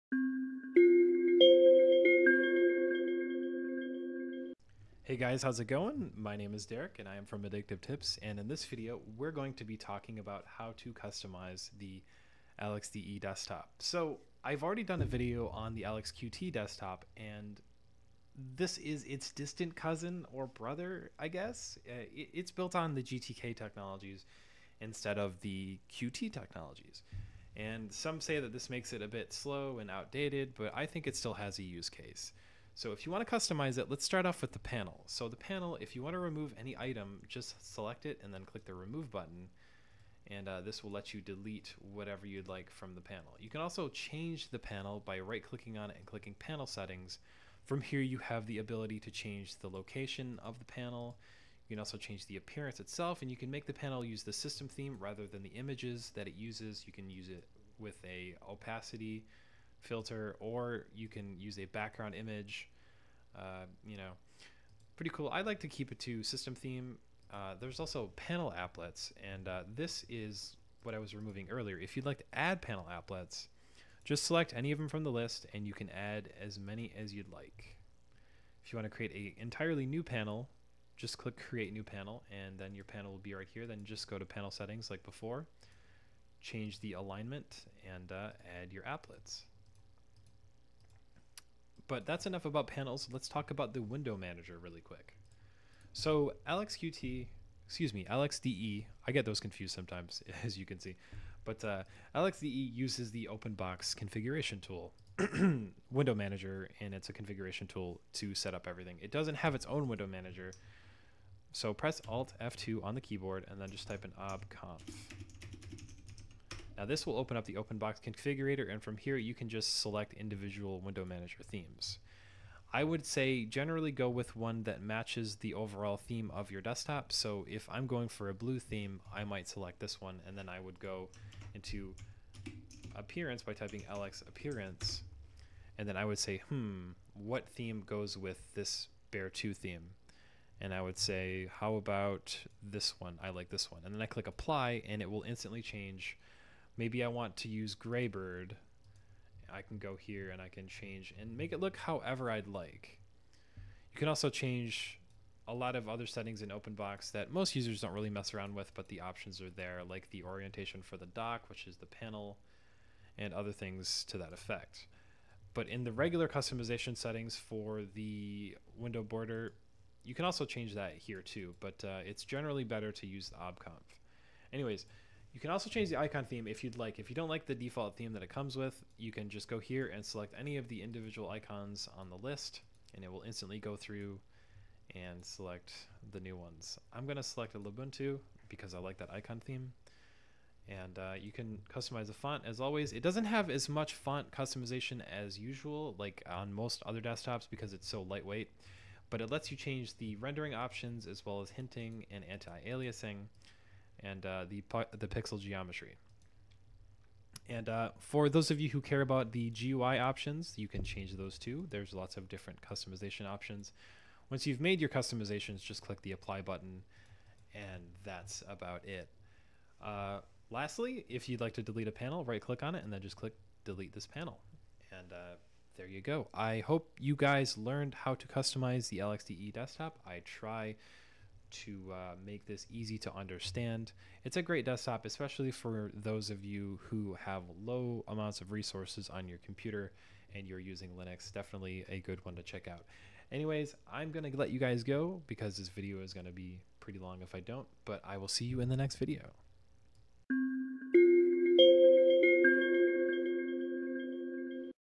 How To Customize The LXDE Desktop
The LXDE desktop environment is one of the best Linux desktops to use if you’re on a PC with low resources. Granted, all of the lightweight desktops out there aspire to be good for low-resource machines.